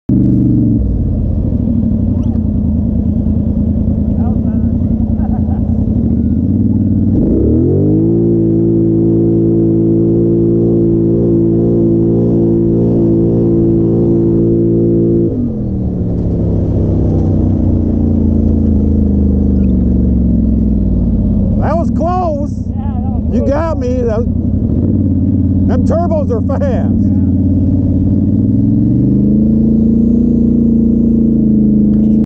RZR Turbo vs 2017 Sportsman XP 1000, Aug 19
Bundy Hill. Drag Strip.
Mountain Dew, My Energy Drink, and Co-Driver, lol.